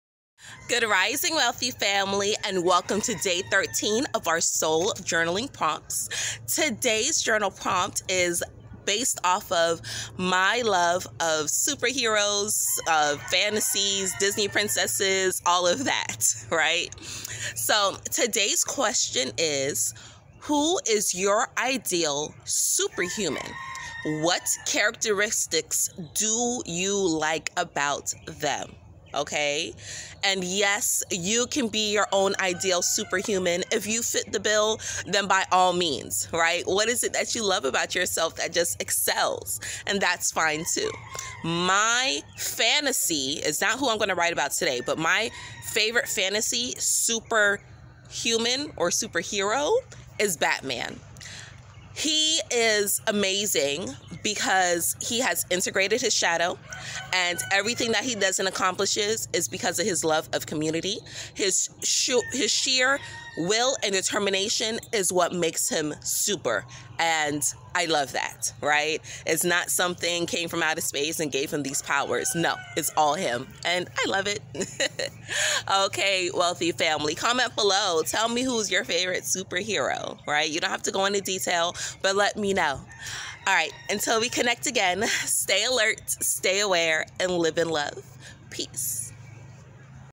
Day 13- Soul Therapy Journaling Prompt
Day 13 question is based off ideals of being a SUPER HUMAN.  Lets jump right in!